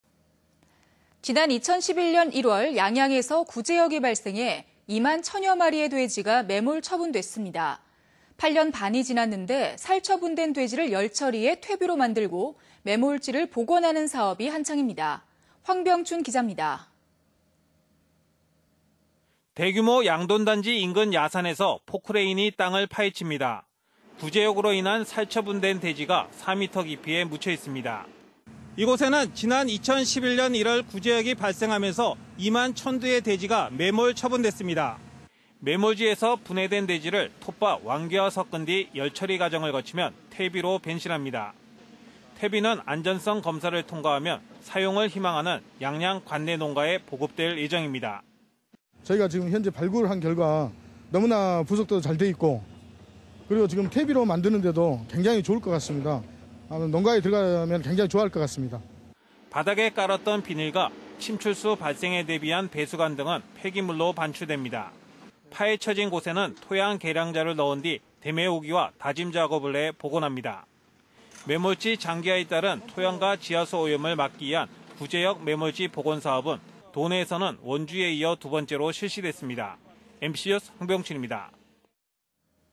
[뉴스리포트]양양 구제역 매몰지 복원사업 한창 190831
지난 2011년 1월 양양에서 구제역이 발생해
2만천여 마리의 돼지가 매몰 처분됐습니다.

8년 반이 지났는데 살처분된 돼지를 열처리해 퇴비로 만들고
매몰지를 복원하는 사업이 한창입니다.